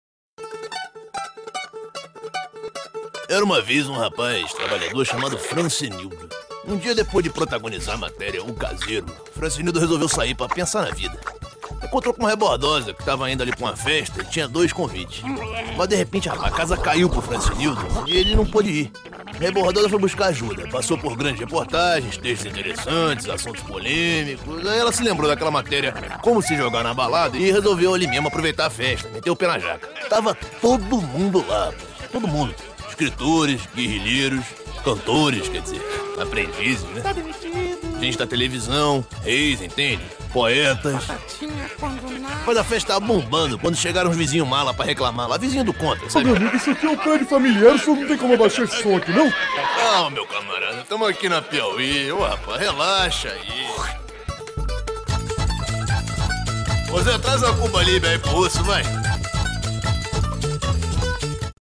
F/Nazca cria filme animado para Revista piauí
Para mostrar a diversidade de assuntos que se encontram na piauí, a revista lança um novo filme institucional, criado pela F/Nazca Saatchi & Saatchi. Por ser tão uma revista muito eclética e com conteúdo diferenciado, não é fácil apresentar em um filme tudo que o leitor pode encontrar na piauí. Até porque cada mês é uma surpresa, não existe padrão. A única regra é que a história, seja ela qual for, precisa ser sempre bem contada. Cabe um perfil de José Dirceu, uma matéria sobre pombos-correio ou quadrinhos cômicos.